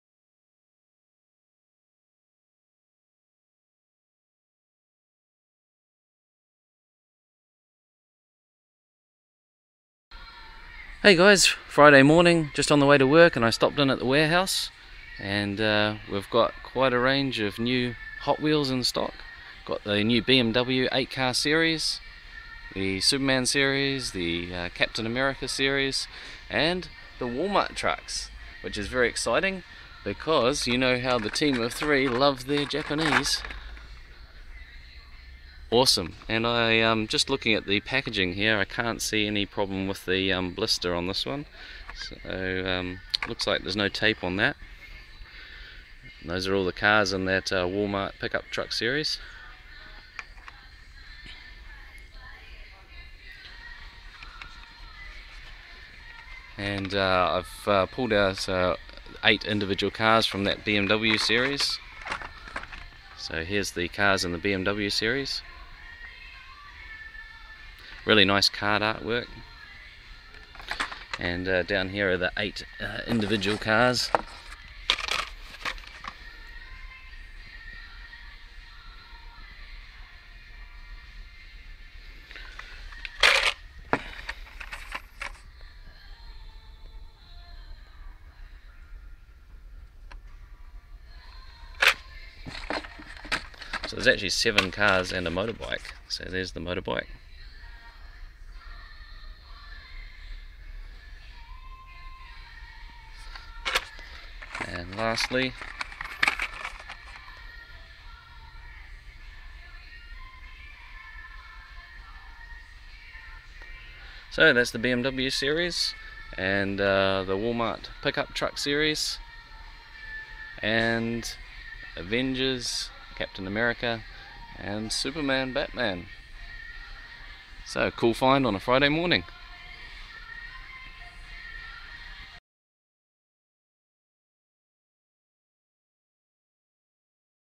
DCTV Lite Ep 60 - BMW 8 Car Set and Hot Wheels Pick Up Trucks
In this video, Cobra finds the new BMW 8 car set, and the Walmart Hot Wheels Pick Up Trucks series.